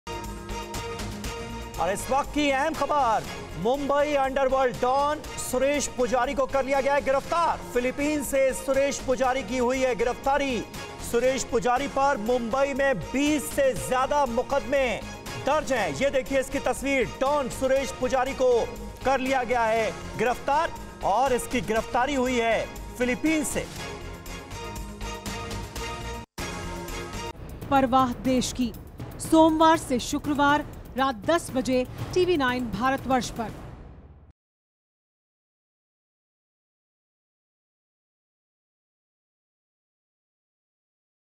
मुंबई अंडरवर्ल्ड डॉन सुरेश पुजारी गिरफ्तार | Mumbai Underworld Don Suresh Pujari Arrested
मुंबई अंडरवर्ल्ड डॉन सुरेश पुजारी को गिरफ्तार कर लिया है. सुरेश पुजारी को फिलीपींस से गिरफ्तार किया गया है.